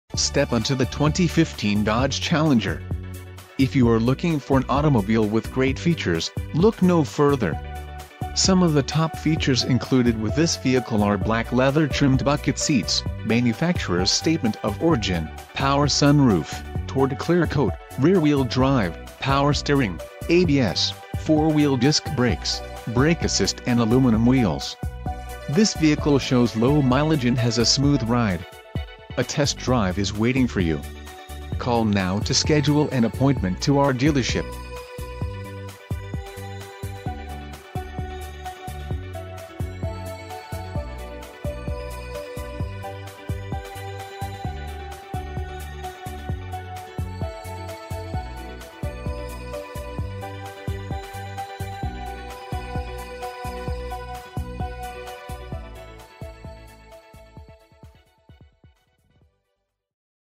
2015 Dodge Challenger SXT Plus - Benny Boyd Andrews - And...
2015 Dodge Challenger SXT Plus Condition: New Mileage: 10 Transmission: 8-Speed Automatic w/OD Exterior Color: Red Doors: 2 Stock: FH758740 Cylinders: 6